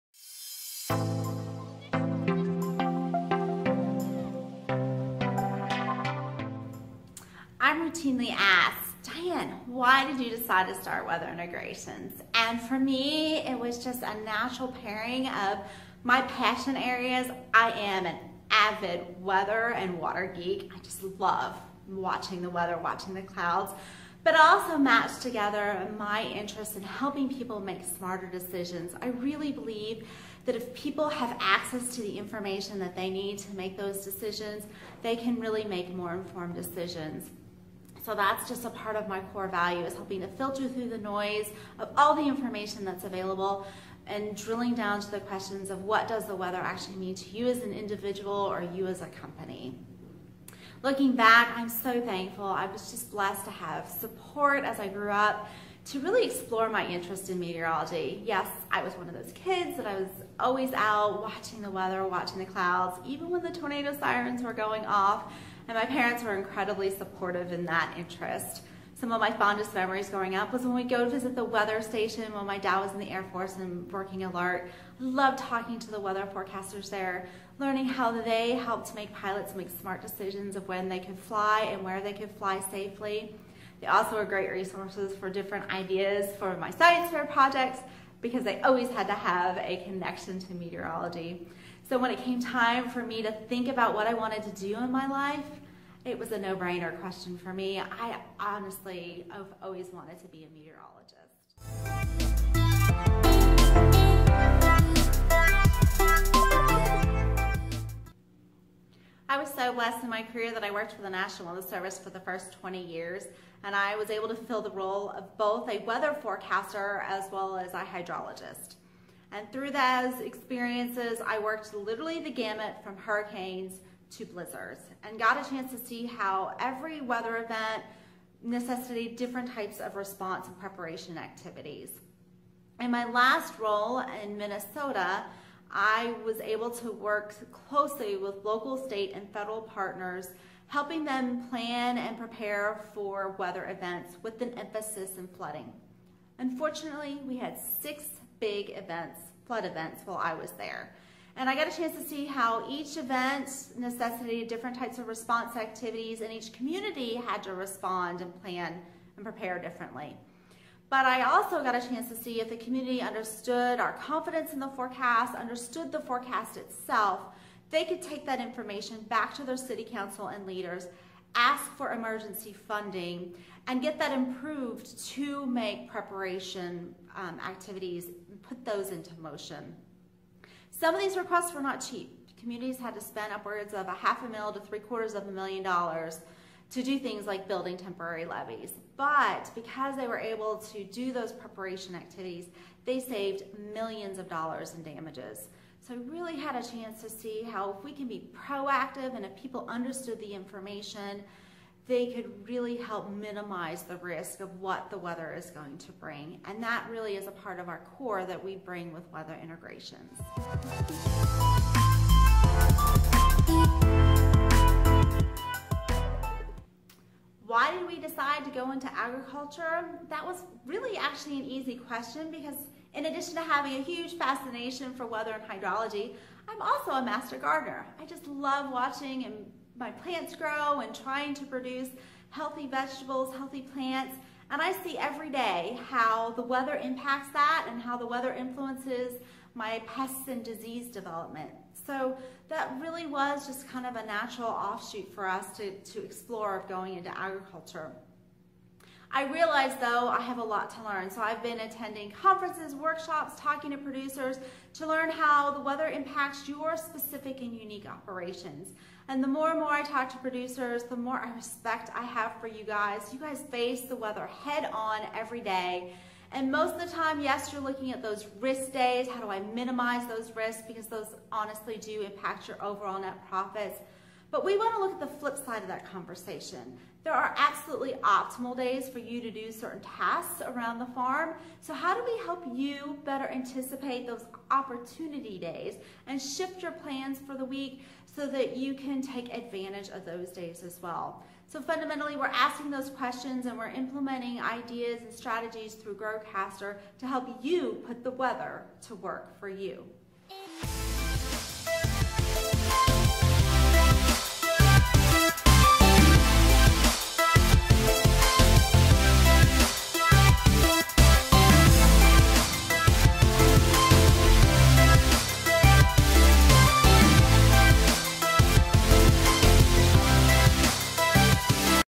Dianes story
Diane Cooper, WxIntegrations CEO, shares about her background, her experiences in working with the National Weather Service and how that laid the foundation for WxIntegrations. She ends the video answering the questions of out of all of the different industry sectors that are impacted by the weather, why did they choose to provide tools and resources for agriculture as their entry market.

song credits